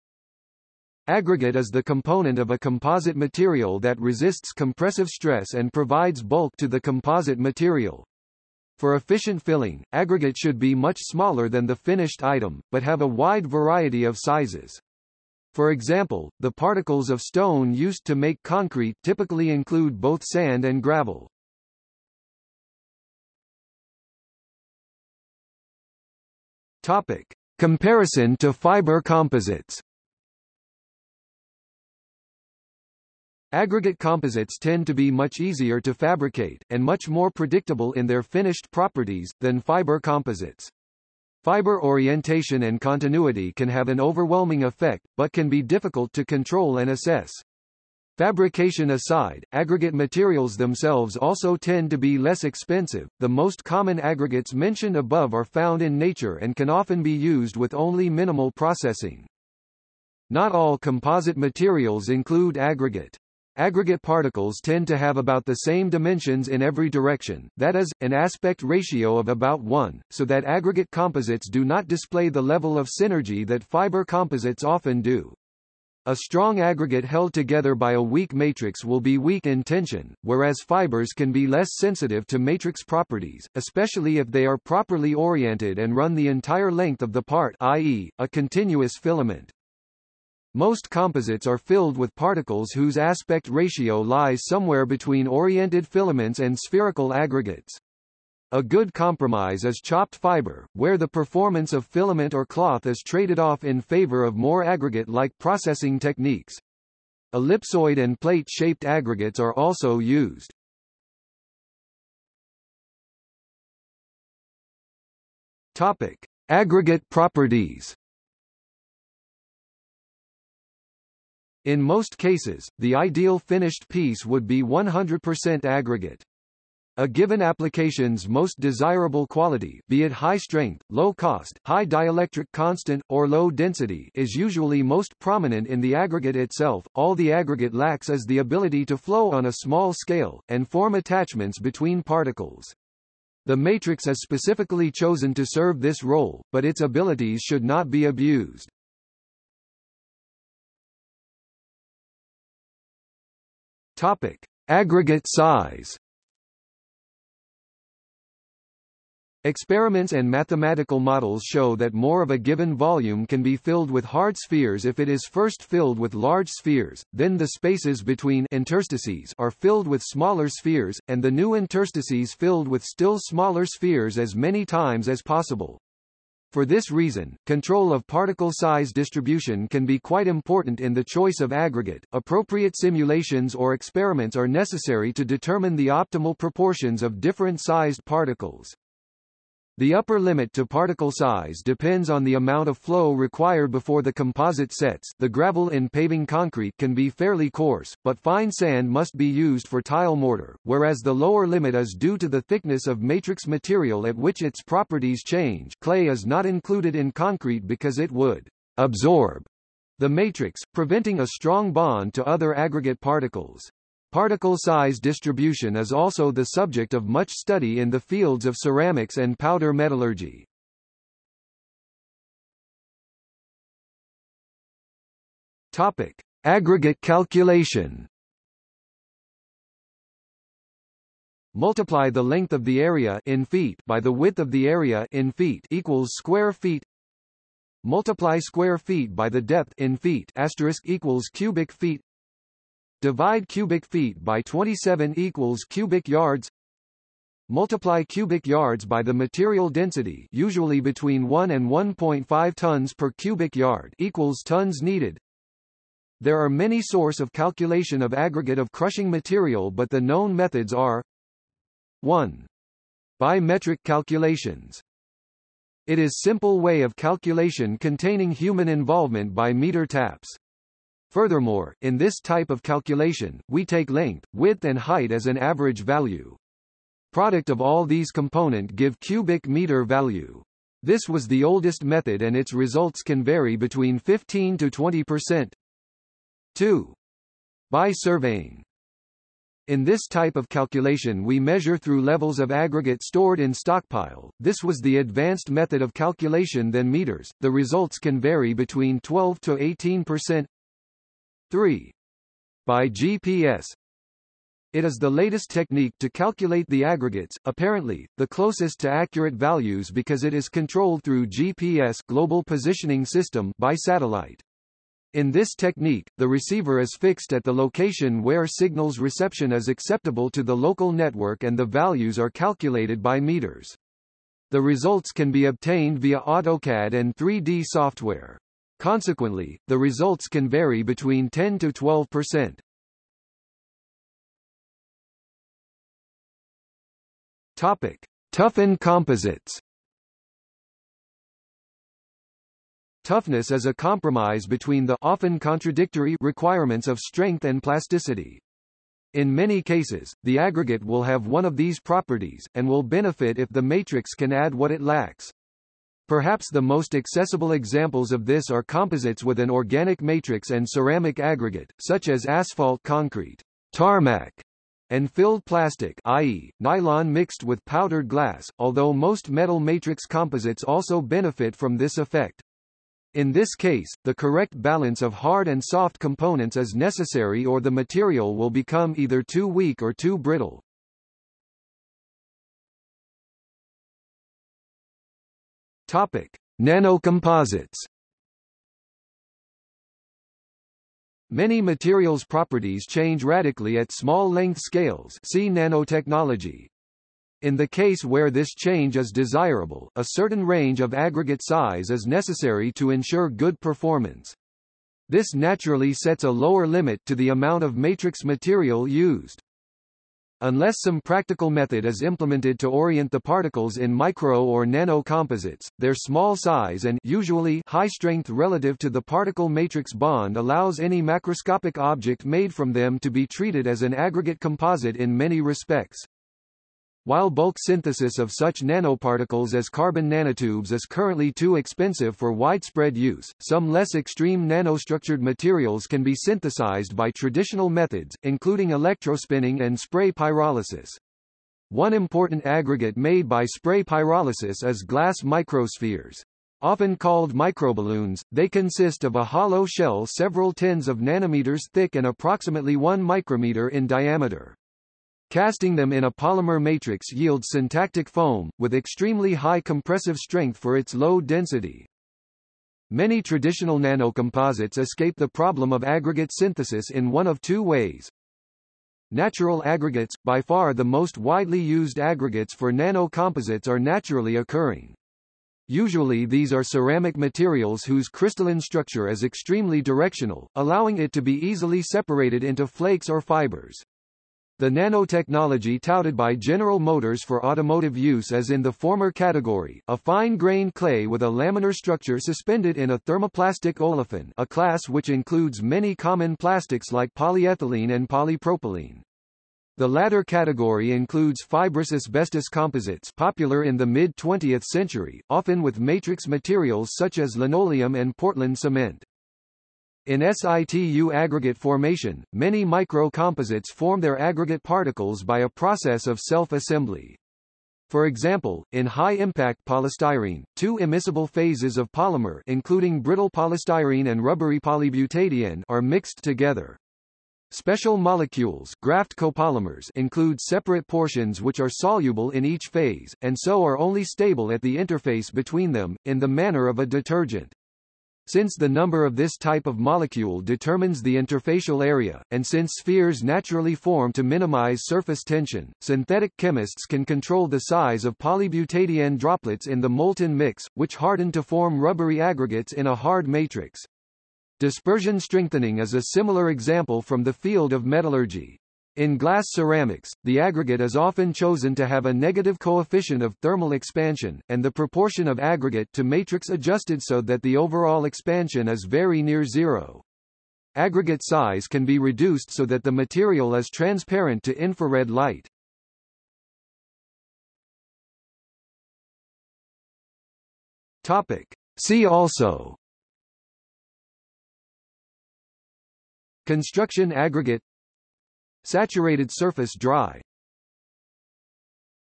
Aggregate (composite) | Wikipedia audio article
00:00:23 1 Comparison to fiber composites
00:01:52 2 Aggregate properties
00:02:30 2.1 Aggregate size
00:03:42 2.2 Aggregate calculation
00:05:34 2.3 Toughened composites
00:06:22 2.4 Nanocomposites
00:09:37 3 See also

- Socrates

SUMMARY
Aggregate is the component of a composite material that resists compressive stress and provides bulk to the composite material. For efficient filling, aggregate should be much smaller than the finished item, but have a wide variety of sizes. For example, the particles of stone used to make concrete typically include both sand and gravel.